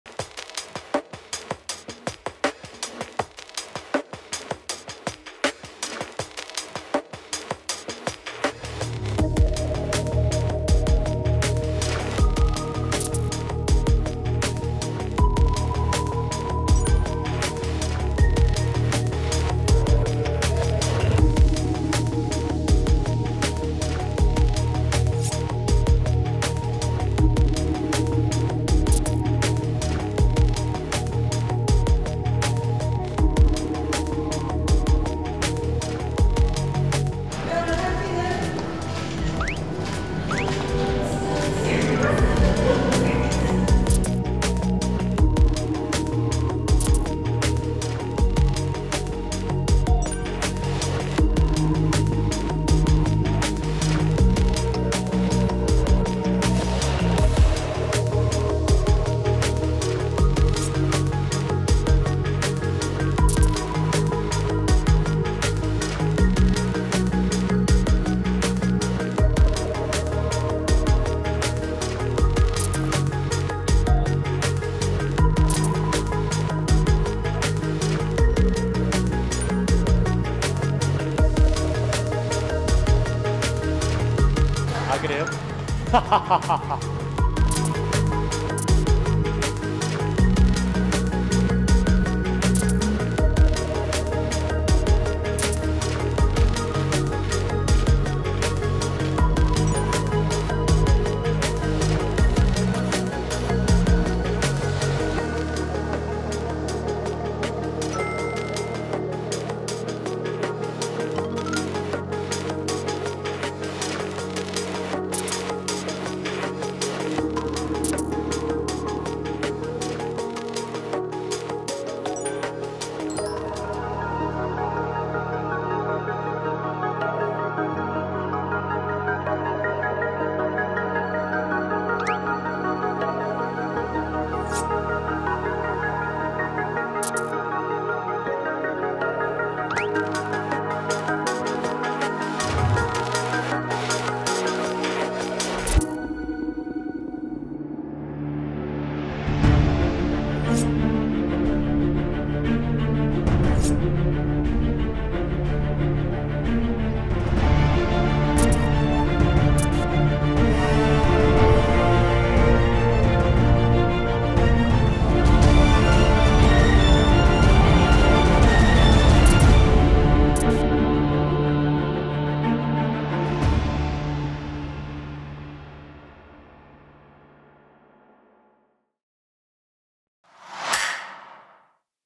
[그리드] 캐릭터 동기화 완. 반전매력 폭발하는 그리드 포스터 촬영 현장 | 디즈니+
장꾸미에 멋쁨✨ 폭발 뭐야...♥
배우들의 반전 매력 가득한
[그리드] 포스터 촬영현장 대공개🙌

[그리드] 2월 16일 디즈니+ 단독 공개까지 D-1 !